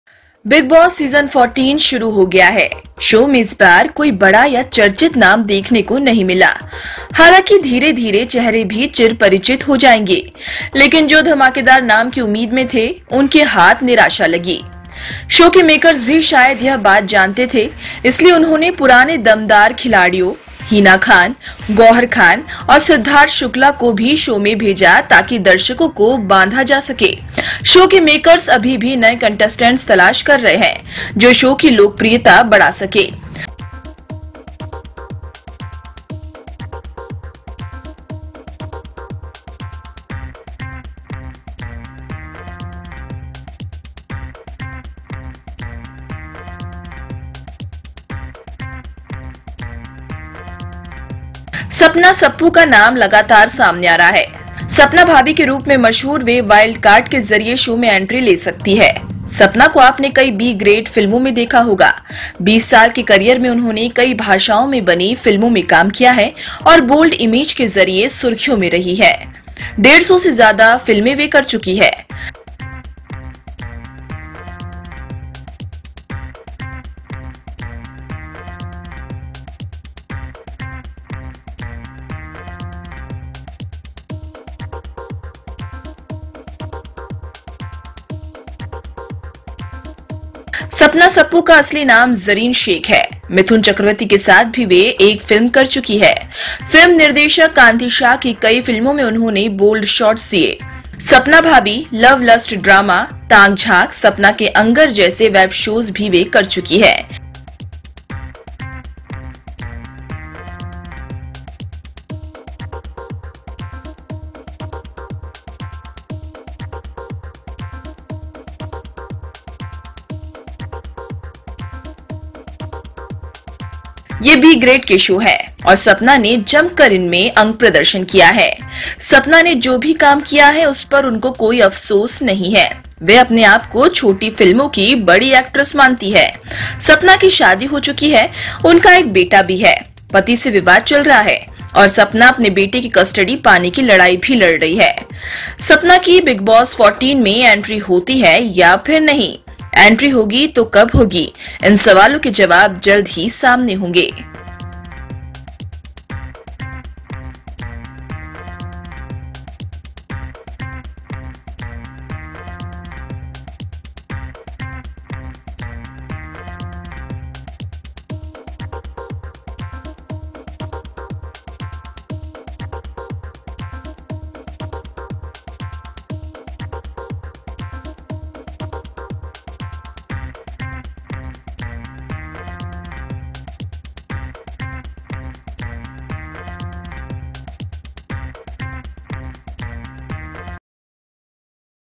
Sapna Bhabhi की Bigg Boss 14 में होगी वाइल्ड कार्ड एंट्री?
बिग बॉस सीज़न 14 (Bigg Boss 14) शुरू हो गया है। शो में इस बार कोई बड़ा या चर्चित नाम देखने को नहीं मिला। हालांकि धीरे-धीरे ये चेहरे भी चिर-परिचित हो जाएंगे, लेकिन जो धमाकेदार नाम की उम्मीद में थे उनके हाथ निराशा लगी।

शो के मेकर्स भी शायद यह बात जानते थे इसलिए उन्होंने पुराने दमदार खिलाड़ियों, हिना खान (Hina Khan), गौहर खान (Gauhar Khan) और सिद्धार्थ शुक्ला (Siddharth Shukla), को भी शो में भेजा ताकि दर्शकों को बांधा जा सके। शो के मेकर्स अभी भी नए कंटेंस्टेंट्स तलाश रहे हैं जो शो की लोकप्रियता बढ़ा सके।

सपना सप्पू (Sapna Sappu) का नाम लगातार सामने आ रहा है। सपना भाभी (Sapna Bhabhi) के रूप में मशहूर वे वाइल्ड कार्ड के जरिये शो में एंट्री ले सकती हैं।

सपना को आपने कई बी-ग्रेड फिल्मों में देखा होगा। 20 साल के करियर में उन्होंने कई भाषाओं में बनी फिल्मों में काम किया है और बोल्ड इमेज के जरिये सुर्खियों में रही हैं। डेढ़ सौ से ज्यादा फिल्में वे कर चुकी हैं।

सपना सप्पू का असली नाम ज़रीना शेख है। मिथुन चक्रवर्ती के साथ भी वे एक फिल्म कर चुकी हैं। फिल्म निर्देशक कांति शाह की कई फिल्मों में उन्होंने बोल्ड शॉट्स ‍दिए। सपना भाभी, लव लस्ट ड्रामा, तांक झांक, सपना के अंगर जैसे वेब शोज़ भी वे कर चुकी हैं। ये बी-ग्रेड के शो हैं और सपना ने जम कर इनमें अंग प्रदर्शन किया है।